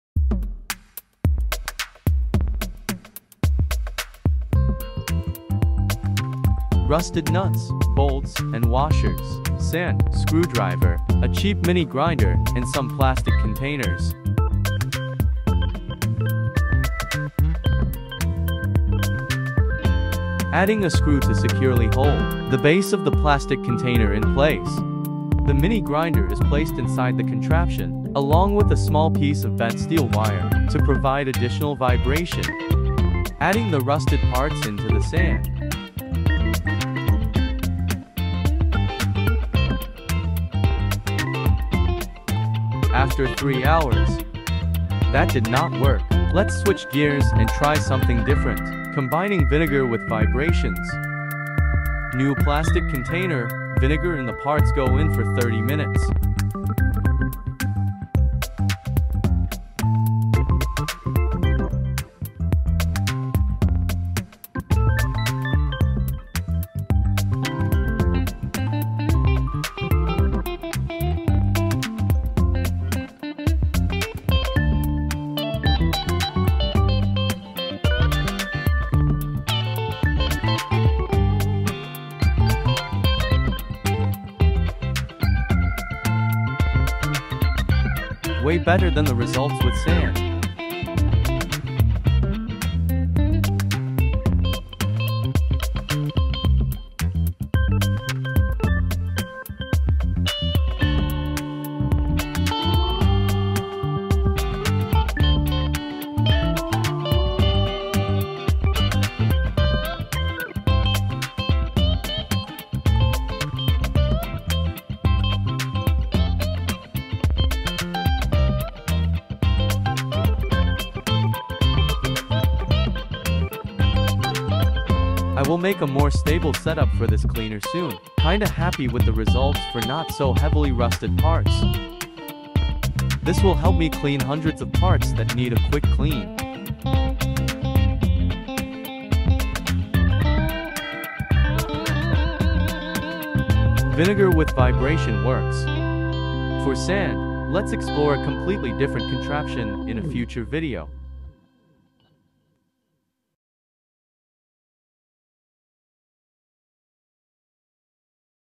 DIY Vibrating Rusted Parts Cleaner - Sand or Vinegar? | Effective Rust Removal
Discover the effectiveness of a homemade vibrating rusted parts cleaner using either sand or vinegar.
In this video, I test out a minimalist setup to determine the best approach.

The setup includes a budget-friendly mini grinder with a bent steel wire for vibrations, using a plastic container as the base and a reusable plastic container as the top part to hold the sand or vinegar.

In my experimentation, I initially tried the sand + vibrations method for a duration of 3 hours with the rusted parts, but it didn't yield the expected results.
However, when I switched to the Vinegar + Vibrations method, the rust removal was remarkably successful in just 30 minutes.

For the Vinegar-based cleaner, I plan to develop a more stable setup suitable for daily use. As for the Sand-based method, I believe a different simple approach may be necessary.